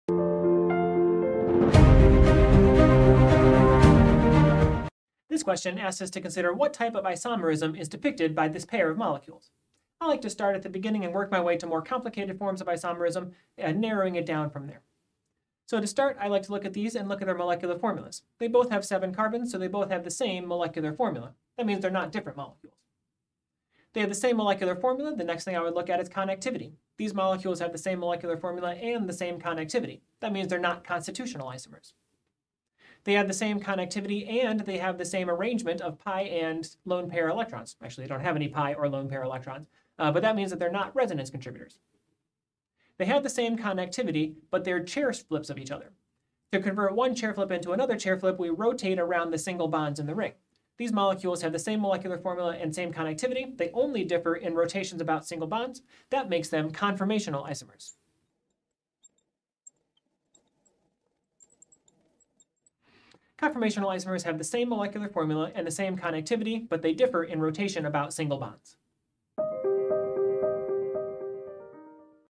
Assessment 6.5(a)
Watch Adam solve Assessment 6.5(a) from the 1st edition of Organic Chemistry: A Learner-Centered Approach by Richard Mullins.